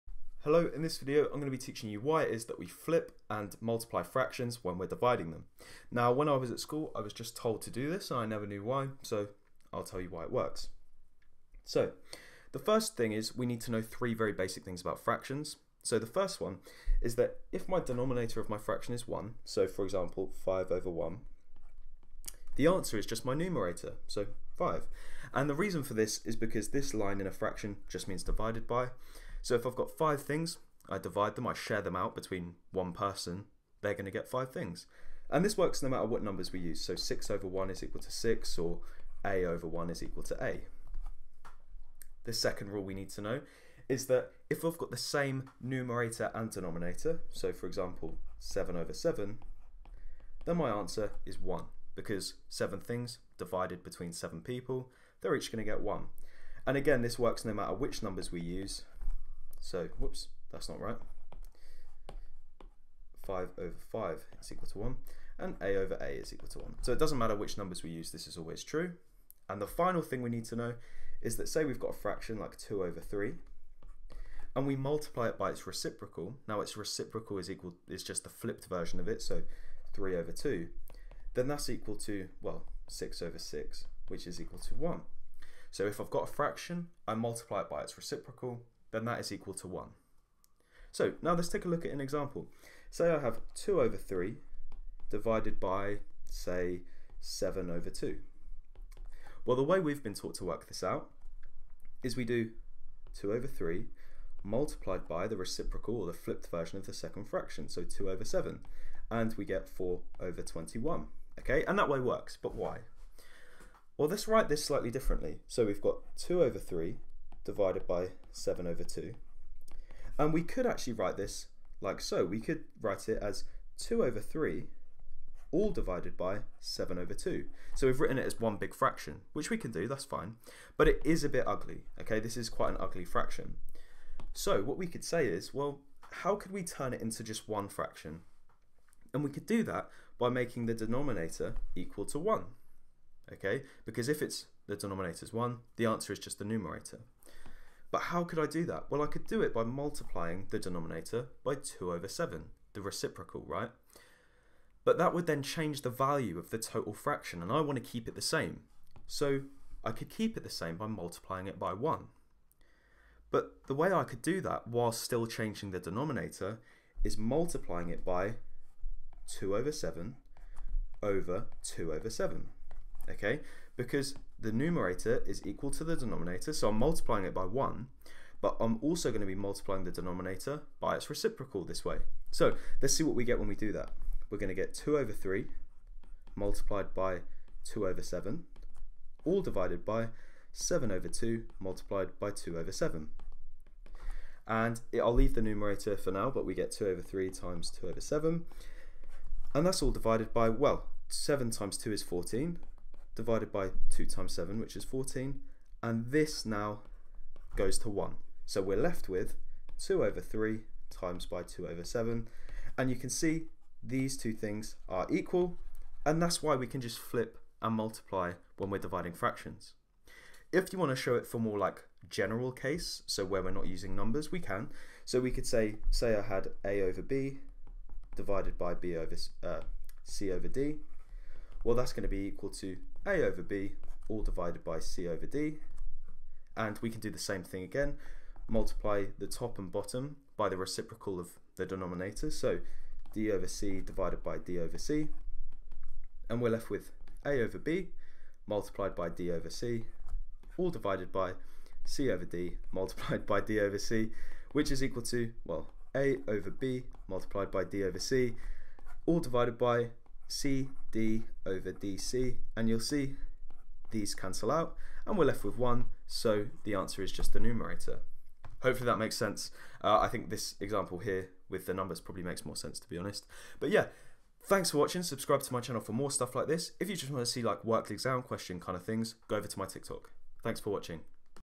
WHY DO WE FLIP & MULTIPLY FRACTIONS WHEN WE DIVIDE? | Why Is That?
1:52 If you want to skip to the explanation!

Have you ever been told to flip and multiply fractions when multiplying them? Why is that? In this video I will explain why!